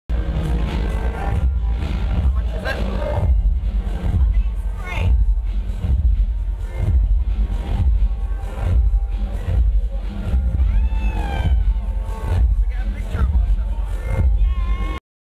Four Tet Curated Brixton All-nighter - What do you like about Four Tet Curated?
O2 asked fans at O2 Academy Brixton: "What do you like about Four Tet Curated?"